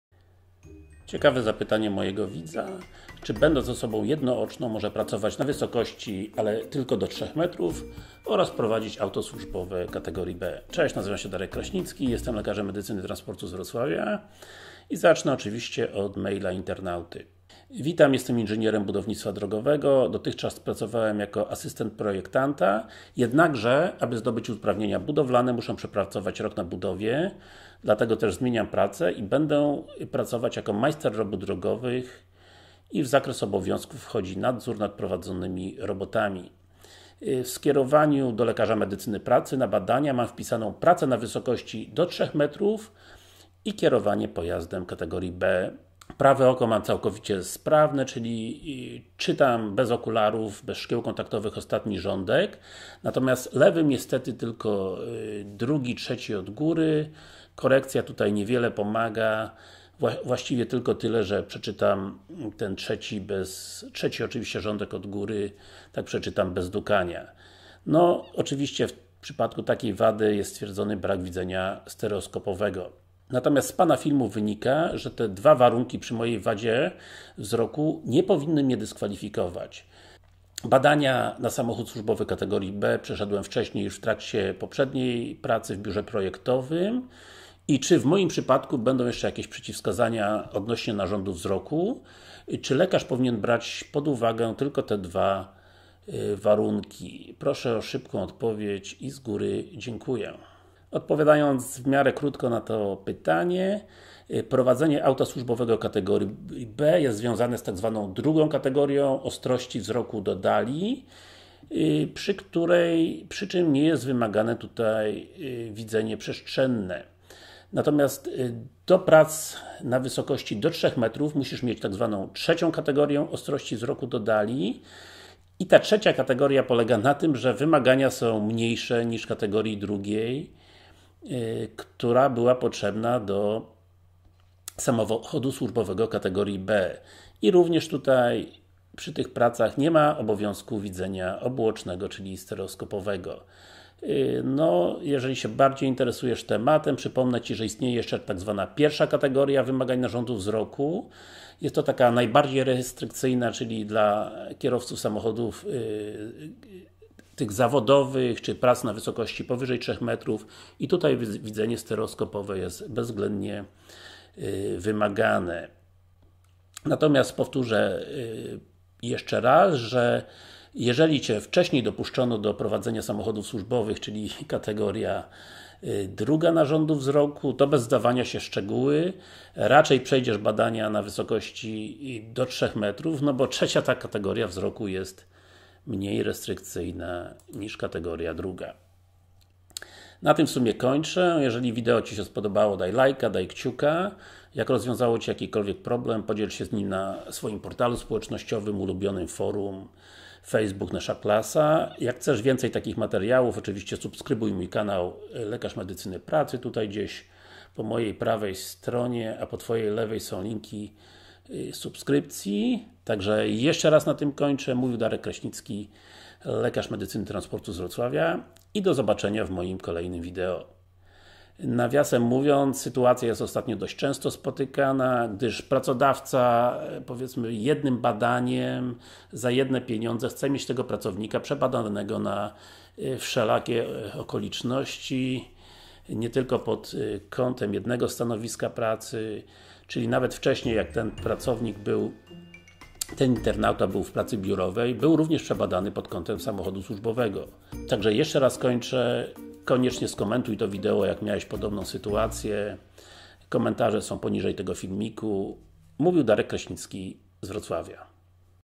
Praca do 3 metrów wysokości i auto służbowe?
Czy mając brak widzenia obuocznego przejdziesz badania lekarskie dopuszczające Cię do pracy na wysokości oraz prowadzenia samochodu służbowego kategorii B?

Dr Dariusz Kraśnicki z Wrocławia serdeczne zaprasza Cię do komentarzy tego przypadku